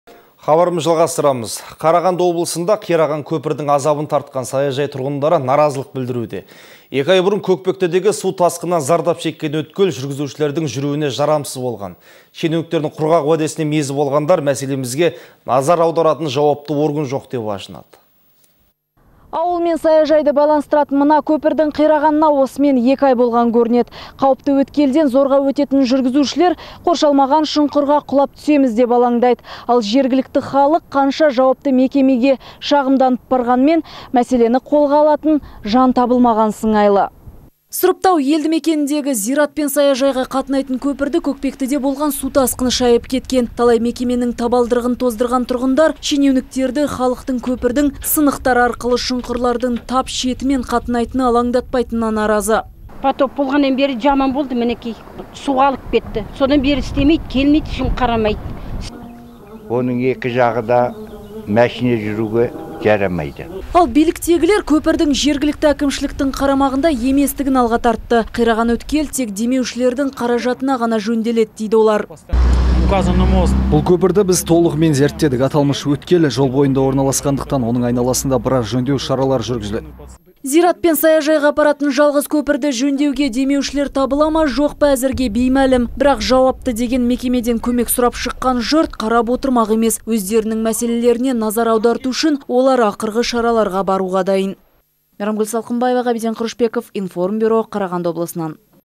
КӨКПЕКТІ КӨМЕККЕ ЗӘРУ
Қарағанды облысында қираған көпірдің азабын тартқан саяжайшылар наразылық білдіруде. Екі ай бұрын Көкпектідегі су тасқынынан зардап шеккен өткел жүргізушілердің жүруіне жарамсыз болған. Шенеуніктердің құрғақ уәдесінен мезі болғандар «мәселемізге назар аударатын жауапты орын жоқ» деп ашынады.
Мейрамгүл Салқымбаева, тілші
 «Ауыл мен саяжайларды байланыстыратын мына көпірдің қирағанына осымен екі ай болған көрінеді. Қауіпті өткелден зорға өтетін жүргізушілер қоршалмаған шұңқырларға құлап түсеміз деп алаңдайды. Ал жергілікті халық жауапты мекемеге қанша шағымданғанымен мәселені қолға алатын жан табылмаған сыңайлы.»
Сұрыптау елдімекеніндегі зират пен саяжайға қатынайтын көпірді Көкпектіде болған су тасқыны шайып кеткен. Талай мекеменің табалдырығын тоздырған тұрғындар шенеуніктерді халықтың көпірдің сынықтары арқылы шұңқырлардың тап шетімен қатынайтыны алаңдатпайтынына наразы.
Нұрғасима Оразбаева, саяжай тұрғыны:
«Потоп болғалы бері жаман болды. Мінекей. Жаман болды. Су алып кетті. Содан бері ешкім келмейді қарамайды. Істесін қарасын деп тілейміз.»
Исабек Рысбек, саяжай тұрғыны:
«Су тасқыны болғаннан кейін осындай жағдай болды. Оның екі жағы да машина жүруге жарамайды.»
Ал биліктегілер көпірдің жергілікті әкімшіліктің қарамағында еместігін алға тартты. «Қираған өткел тек демеушілердің қаражатына ғана жөнделеді»,-дейді олар.
Қайрат Теміржанов, Қарағанды қаласы автомобильді жолдар секторының меңгерушісі:
«Бұл көпірді біз толығымен зерттедік. Аталмыш  өткел жол бойында орналасқандықтан, оның айналасында біраз жөндеу шаралары жүргізіледі. Жақын күндері ұсақ тас төселетін болады. Ал көпірдің өзіне біз тиіспейміз.»
Зират пен саяжайға апаратын жалғыз көпірді жөндеуге демеуші табыла ма жоқ па әзірге беймәлім. Бірақ жауапты деген мекемеден көмек сұрап шыққан жұрт қарап отырмақ емес. Өздерінің мәселелеріне назар аударту үшін олар ақырғы шараларға баруға дайын.